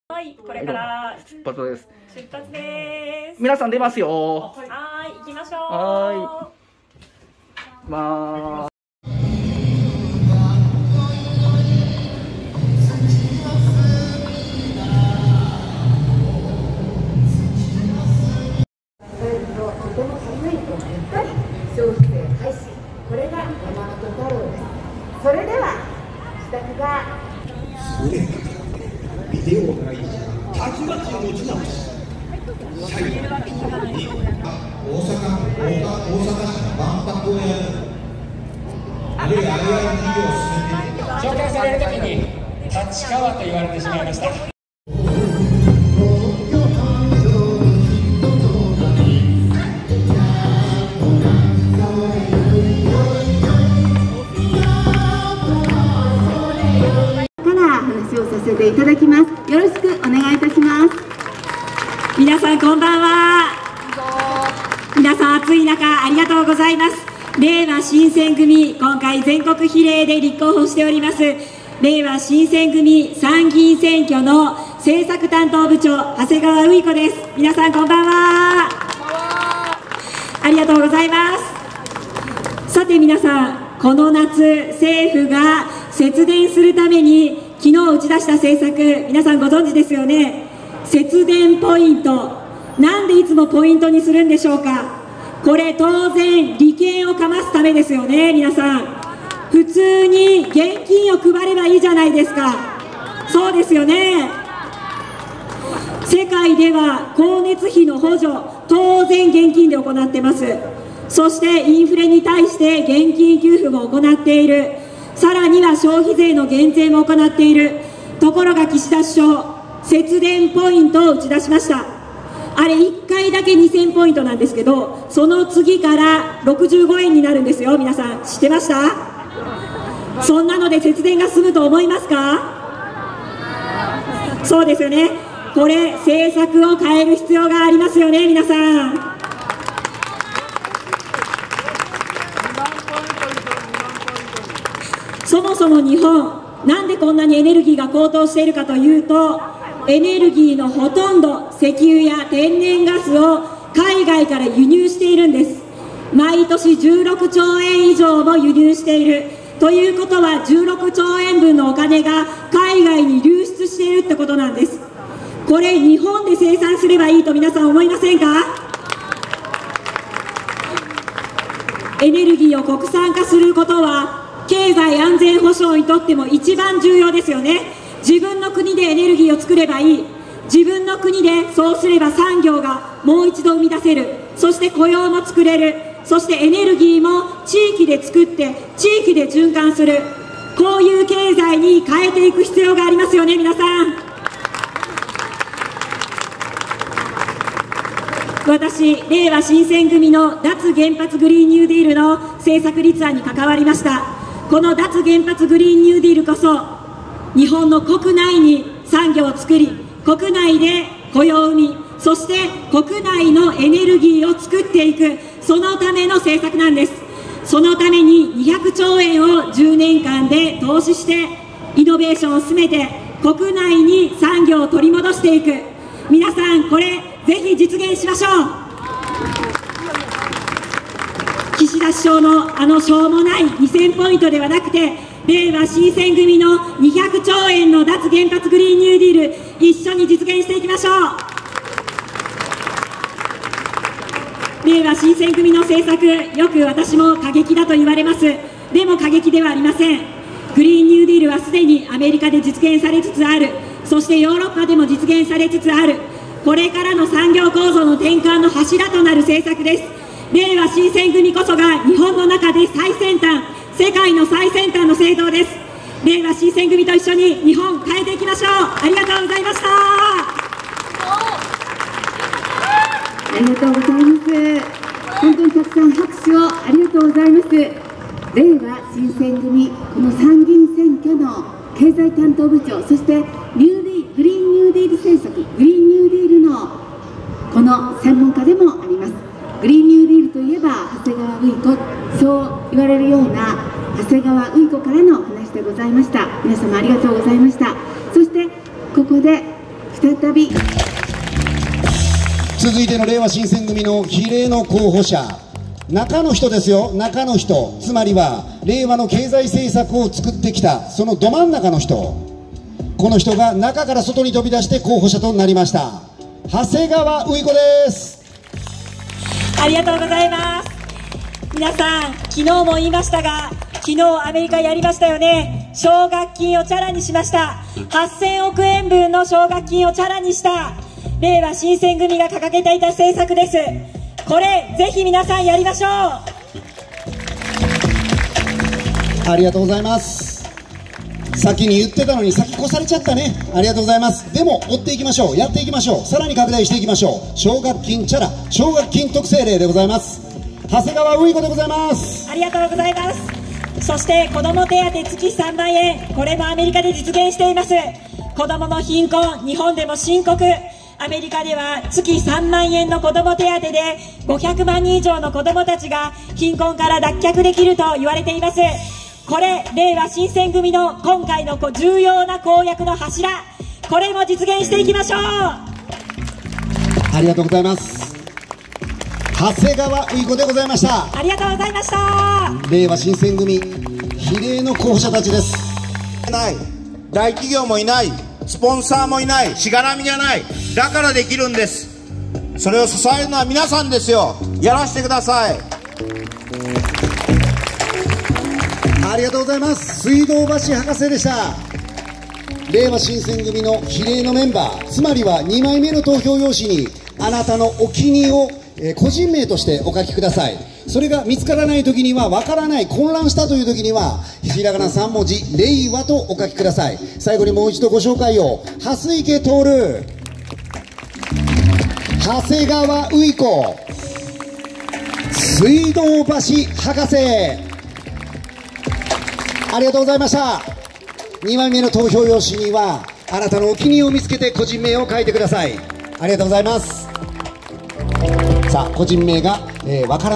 長谷川ういこ in秋葉原盆踊り大会
れいわ新撰組比例区候補の長谷川ういこが、2022/06/25、秋葉原で開催されたれいわ新撰組の盆踊り大会に参加しました。その盛り上がりを体感して下さい（昨日、同様のものをアップしていましたが、タイトルに誤字がありましたので、改めて正しいものをアップいたします）